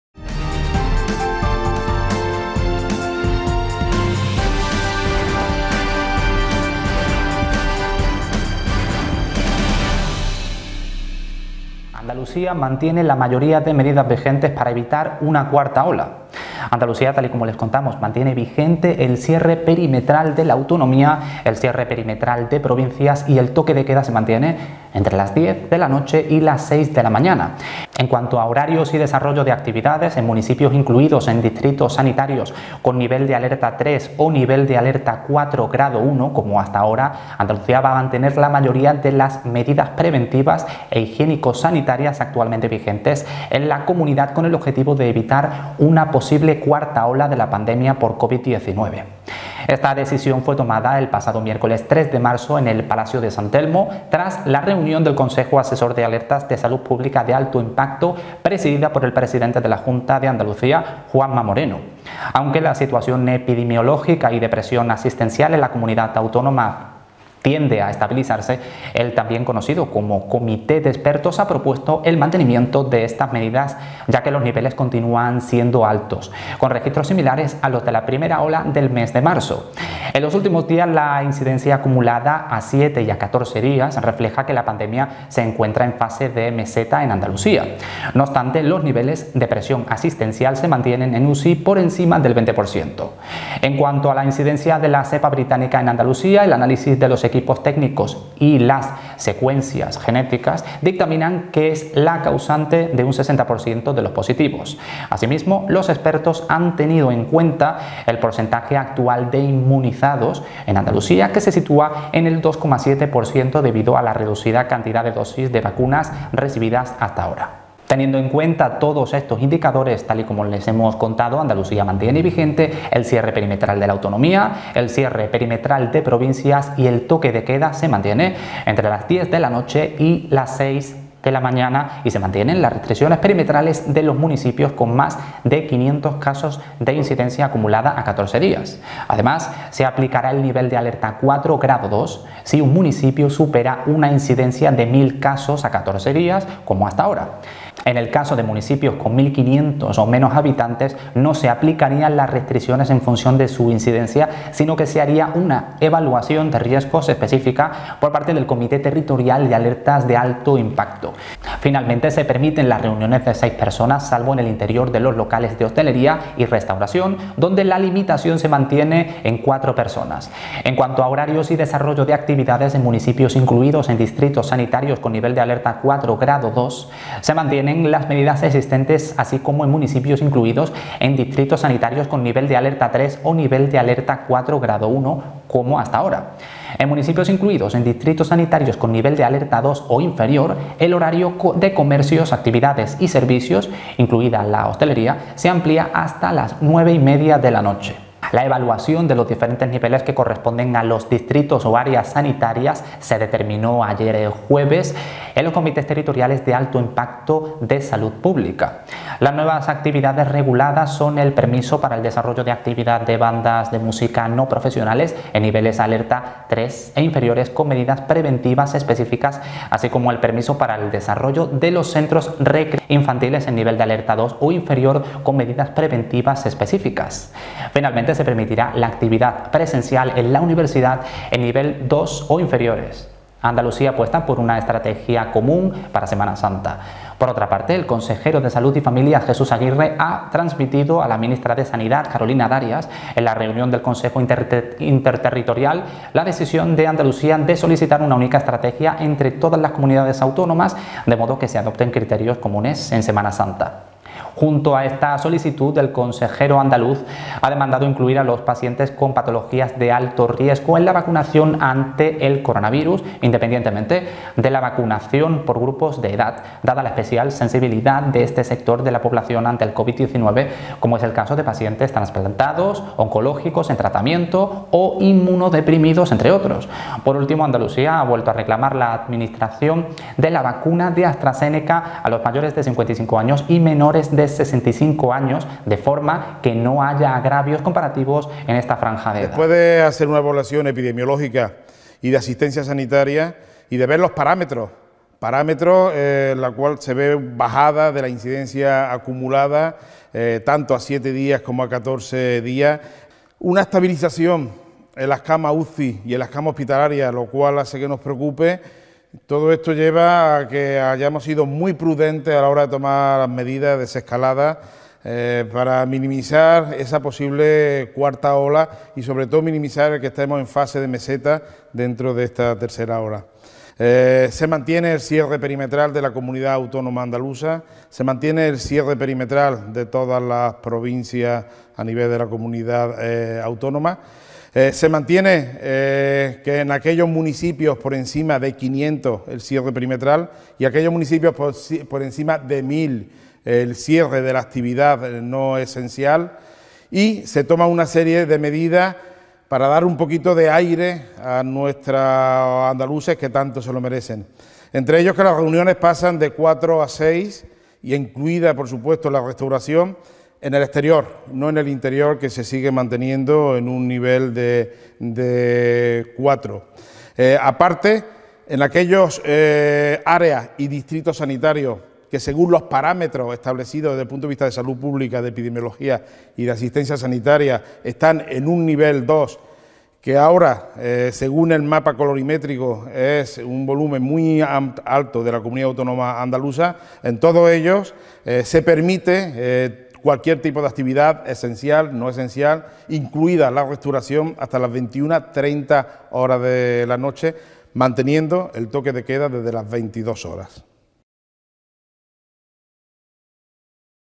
ANDALUCÍA MANTIENE LA MAYORÍA DE MEDIDAS VIGENTES PARA EVITAR UNA CUARTA OLA
Andalucía mantiene vigente el cierre perimetral de la autonomía, el cierre perimetral de provincias y el toque de queda se mantiene de 22.00 a 06.00 horas. En cuanto a horarios y desarrollo de actividades, en municipios incluidos en distritos sanitarios con nivel de alerta 3 o nivel de alerta 4 grado 1, como hasta ahora Andalucía va a mantener la mayoría de las medidas preventivas e higiénico sanitarias actualmente vigentes en la comunidad con el objetivo de evitar una posible cuarta ola de la pandemia por Covid-19. Esta decisión fue tomada el pasado miércoles 3 de marzo en el Palacio de San Telmo, tras la reunión del Consejo Asesor de Alertas de Salud Pública de Alto Impacto, presidida por el presidente de la Junta, Juanma Moreno. Aunque la situación epidemiológica y de presión asistencial en la comunidad autónoma tiende a estabilizarse, el también conocido como Comité de Expertos ha propuesto el mantenimiento de estas medidas ya que los niveles continúan siendo altos, con registros similares a los de la primera ola del mes de marzo. En los últimos días, la incidencia acumulada a 7 y a 14 días refleja que la pandemia se encuentra en fase de meseta en Andalucía. No obstante, los niveles de presión asistencial se mantienen en UCI por encima del 20%. En cuanto a la incidencia de la cepa británica en Andalucía, el análisis de los equipos técnicos y las secuencias genéticas dictaminan que es la causante de un 60% de los positivos. Asimismo, los expertos han tenido en cuenta el
porcentaje actual de inmunizados en Andalucía que se sitúa en el 2,7%, debido a la reducida cantidad de dosis de vacunas recibidas hasta ahora. Teniendo en cuenta todos estos indicadores, tal y como les hemos contado, Andalucía mantiene vigente el cierre perimetral de la autonomía, el cierre perimetral de provincias y el toque de queda se mantiene de 22.00 a 06.00 horas y se mantienen las restricciones perimetrales de los municipios con más de 500 casos de incidencia acumulada a 14 días. Además, se aplicará el Nivel alerta 4 grado 2 si un municipio supera una incidencia de 1.000 casos a 14 días, como hasta ahora. En el caso de municipios con 1.500 o menos habitantes no se aplicarían las restricciones en función de su incidencia sino que se haría una evaluación de riesgos específica por parte del Comité Territorial de Alertas en Salud Alto Impacto. Finalmente, se permiten las reuniones de 6 personas, salvo en el interior de los locales de hostelería y restauración, donde la limitación se mantiene en 4 personas. En cuanto a horarios y desarrollo de actividades, en municipios incluidos en distritos sanitarios con nivel de alerta 4 grado 2, se mantienen las medidas existentes así como en municipios incluidos en distritos sanitarios con nivel de alerta 3 o nivel de alerta 4 grado 1, como hasta ahora. En municipios incluidos en distritos sanitarios con nivel de alerta 2 o inferior, el horario de comercios, actividades y servicios (incluida la hostelería) se amplía hasta las 21.30 horas. La evaluación de los diferentes niveles que corresponden a los distritos o áreas sanitarias se determinará mañana en los Comités Territoriales de Alto Impacto en Salud Pública. Las nuevas actividades reguladas son el permiso para el desarrollo de actividad de bandas de música no profesionales en niveles alerta 3 e inferiores con medidas preventivas específicas así como el permiso para el desarrollo de los centros recreativos infantiles en nivel de alerta 2 o inferior con medidas preventivas específicas. Finalmente, se permitirá la actividad presencial en la Universidad en nivel 2 o inferiores. Andalucía apuesta por una estrategia común para Semana Santa. Por otra parte, el consejero de Salud y Familias, Jesús Aguirre, ha transmitido a la ministra de Sanidad, Carolina Darias, en la reunión del Consejo Interterritorial la decisión de Andalucía de solicitar una única estrategia entre todas las comunidades autónomas "de modo que se adopten criterios comunes en Semana Santa". Junto a esta solicitud, el consejero andaluz ha demandado incluir a los pacientes con patologías de alto riesgo en la vacunación ante el coronavirus, independientemente de la vacunación por
grupos de edad, "dada la especial sensibilidad de este sector de la población ante el Covid-19, como es el caso de pacientes trasplantados, oncológicos en tratamiento o inmunodeprimidos, entre otros". Por último, Andalucía ha vuelto a reclamar la administración de la vacuna de Astrazeneca a los mayores de 55 años y menores de 65 años "de forma que no haya agravios comparativos en esta franja de edad".